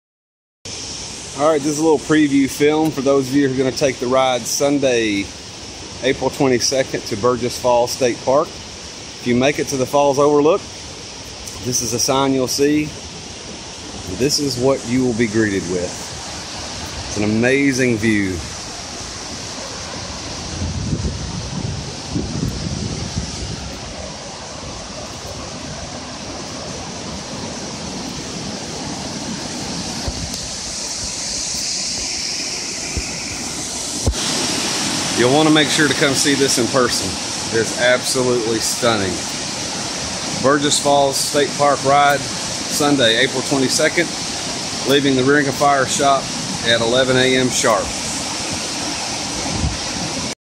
Burgess Falls State Park Ride Preview
This is a short preview video for the 4/22/12 Burgess Falls State Park Ride for the ring of Fire HOG Chapter. This video was uploaded from an Android phone.